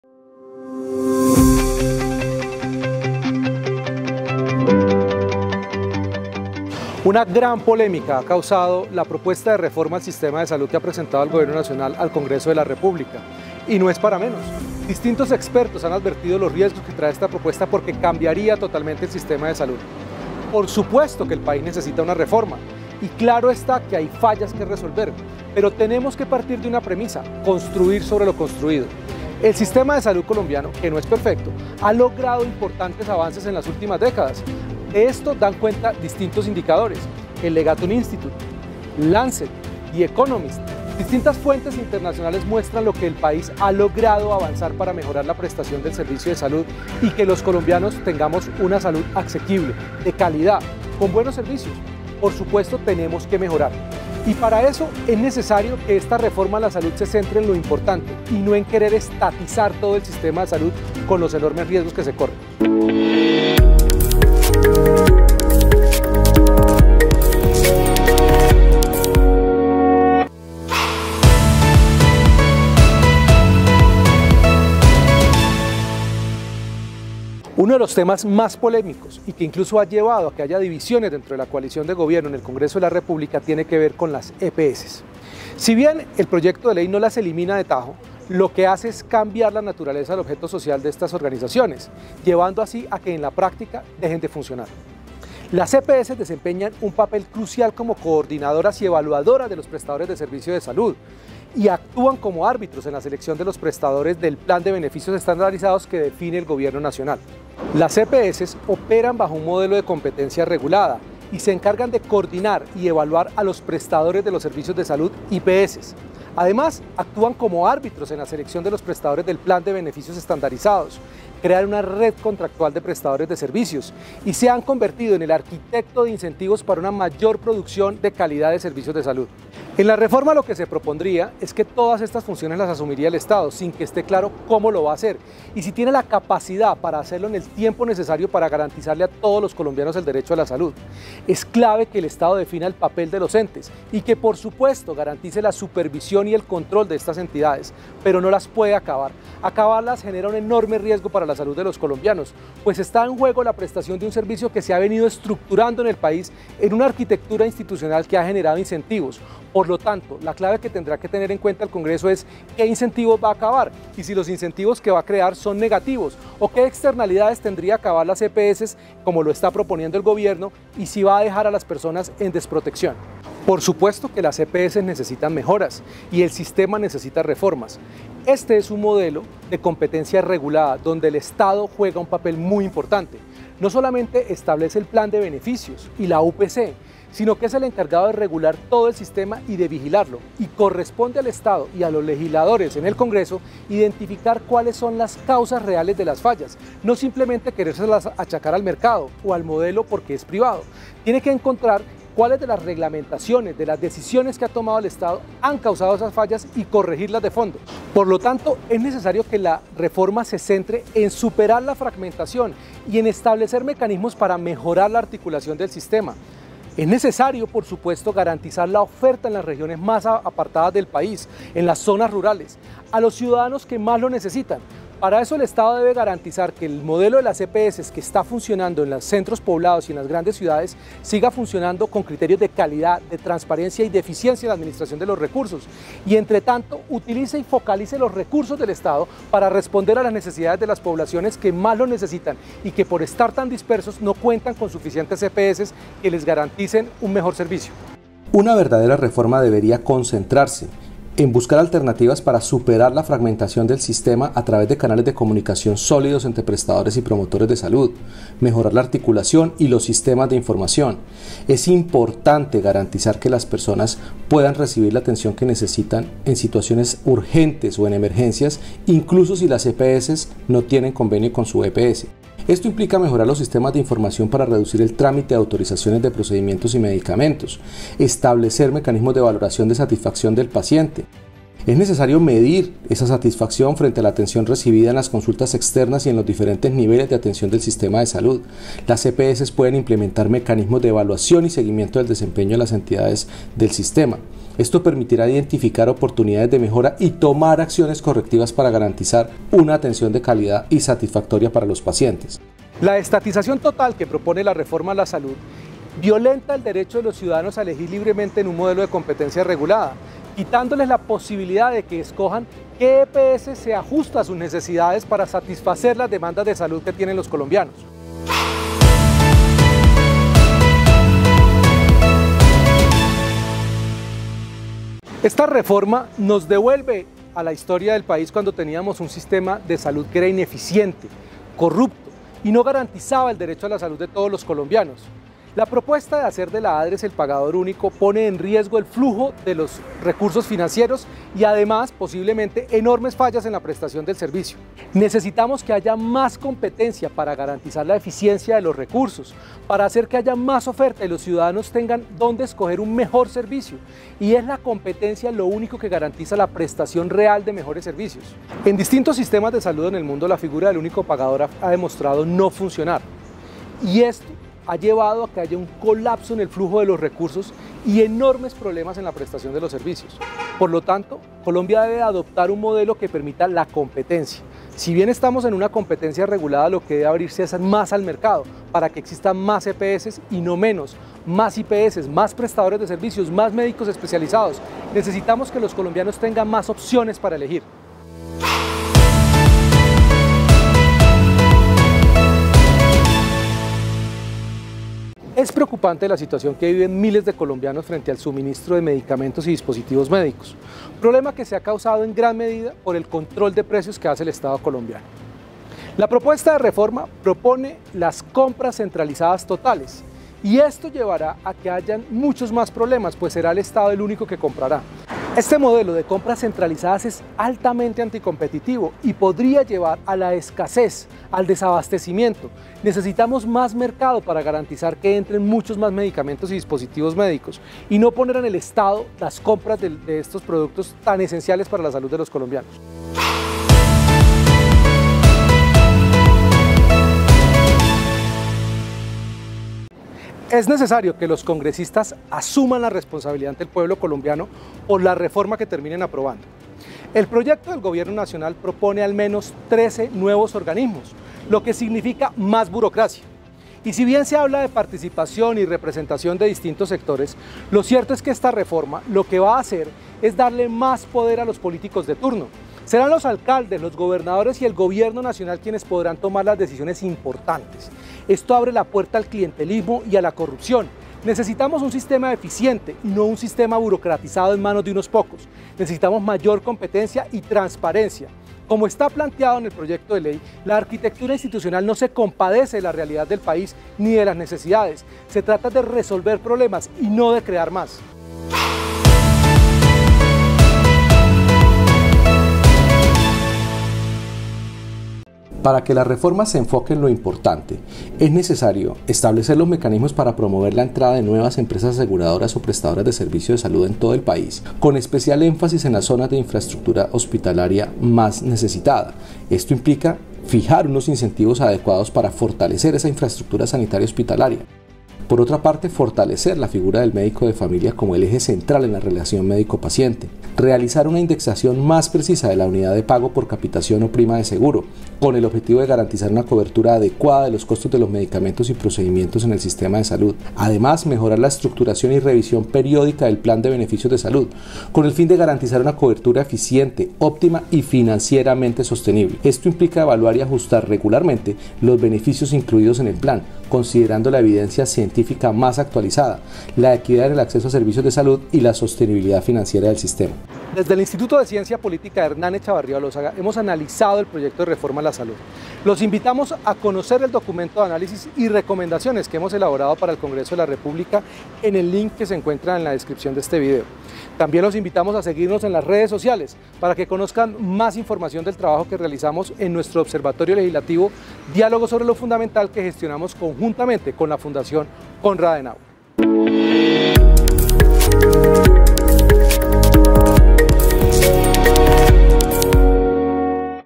¿La reforma de salud mejorará la atención médica en Colombia?
Se discute en el Congreso una reforma a la salud con el objetivo de mejorar el actual sistema de salud colombiano. Sin embargo, varios puntos de la propuesta generan preocupación: el papel de las EPS en este nuevo sistema, el papel de la Adres (Administradora de los Recursos del Sistema General de Seguridad Social en Salud) como único pagador, los obstáculos de tener un tarifario único y los riesgos de burocratización del sistema.

Desde el ICP, junto con @fundacionkonradadenauerofi3140, presentamos las siguientes recomendaciones que los congresistas deben considerar para hacer una reforma a la salud que mejore los problemas del sistema sin acabar con todo lo ya conseguido. ¿Necesitamos que el sistema sea controlado y administrado completamente por el Estado? ¿Tiene cabida las EPS y el sector privado en general para garantizar la calidad de la salud en el país? Estas y más preguntas son respondidas en este video análisis.